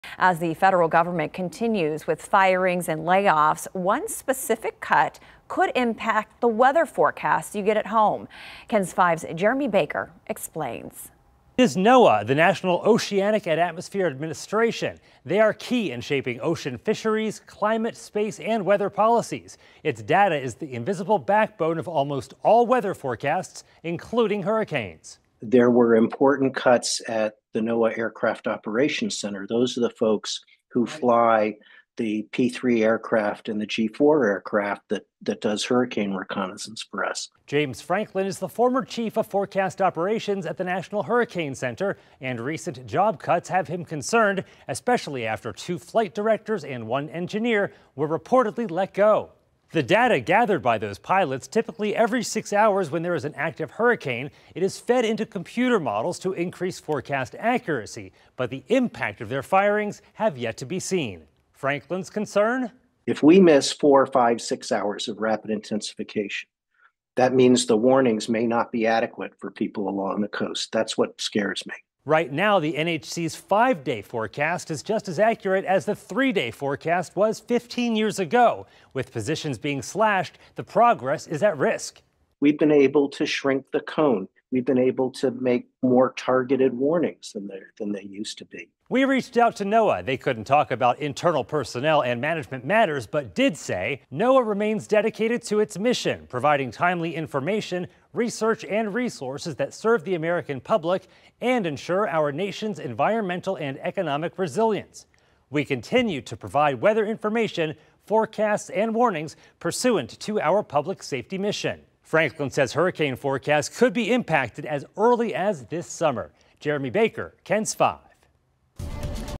Hurricane forecasters express concern over NOAA job cuts impact
The jobs lost could impact the weather forecasts you get at home.